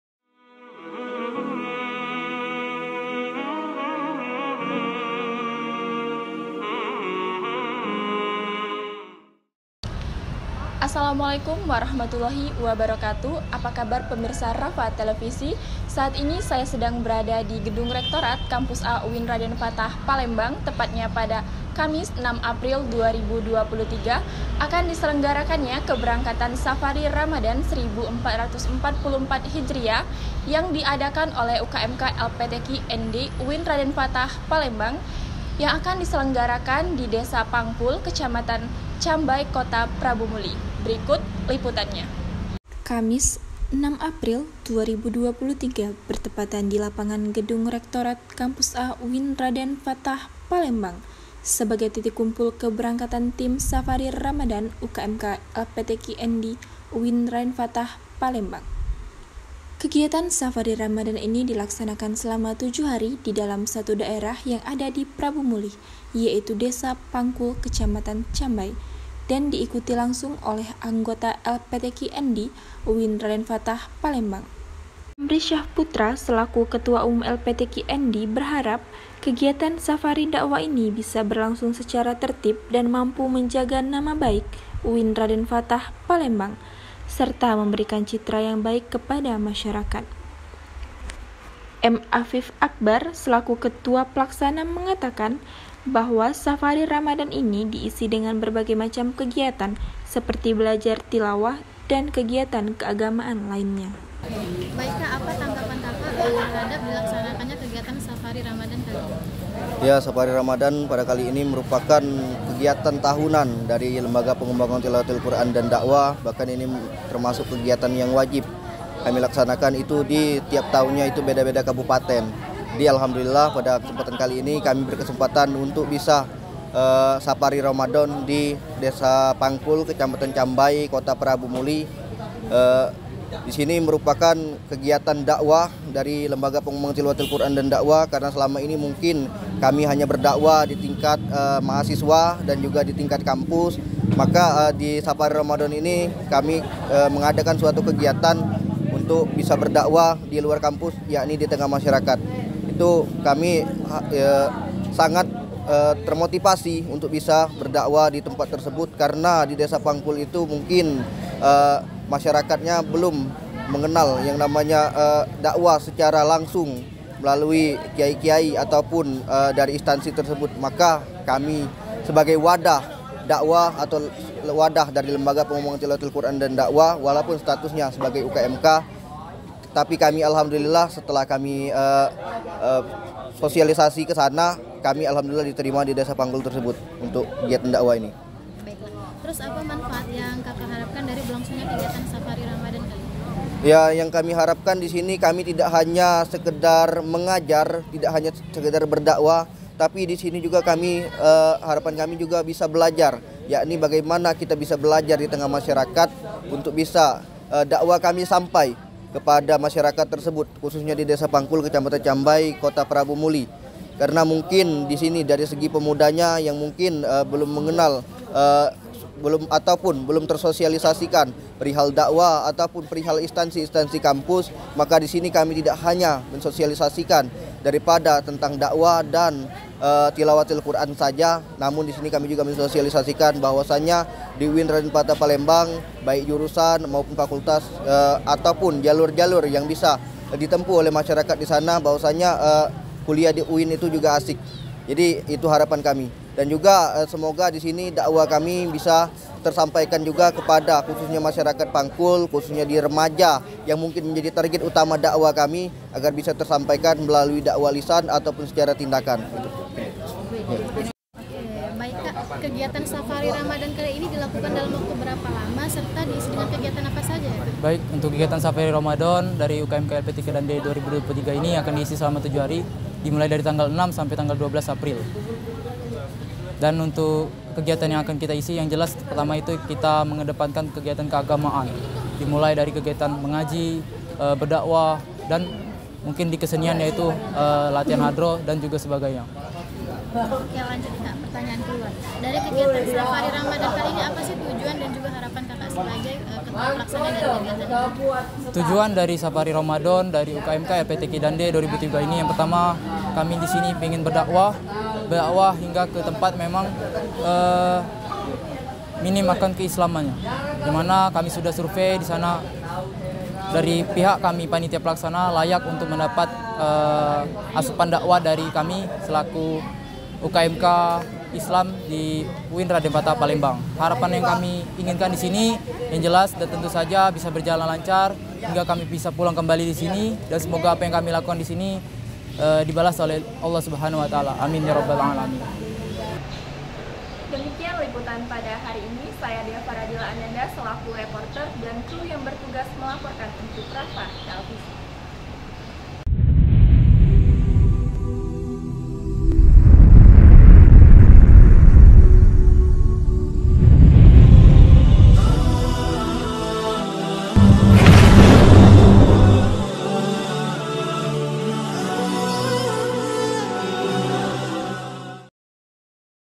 RAFA NEWS || Safari Ramadhan 1444 H UKMK LPTQ&D UIN Raden Fatah Palembang
Kamis, 6 April 2023 bertepatan di Lapangan Gedung Rektorat Kampus A UIN Raden Fatah Palembang sebagai titik kumpul Keberangkatan Tim Safari Ramadhan UKMK LPTQ&D.

Kegiatan safari ramadhan ini dilaksanakan selama 7 hari di salah satu daerah yang ada di Prabumulih yaitu di Desa Pangkul, Cambai. Dan diikuti langsung oleh anggota LPTQ&D UIN Raden Fatah

Amri Syah Putra selaku ketua umum LPTQ&D berharap kegiatan safari dakwah ini bisa berlangsung secara tertib dan mampu menjaga nama baik UIN Raden Fatah Palembang, serta memberikan citra yang baik pada masyarakat.

M. Afif Akbar, selaku ketua pelaksana mengatakan, bahwa safari ramadhan ini diisi dengan berbagai macam kegiatan seperti belajar tilawah dan kegiatan keagamaan lainnya.

Pengawas : Muhammad Randicha Hamandia, M.Sos
Produser : Ahmad Agung Hadly
Adpro : 1.Annisa Adelia
            2.Agil Salsabila
            3.Salsabila Jandisha

Ast. News : Novelin Permanda

[CREW YANG BERTUGAS] :
1. Reporter : Dea Faradila
2. Ass. Reporter : Eltisa Ati
3. Cameraman : M.Abdan Erba
4. Editor : M. Abdan Erba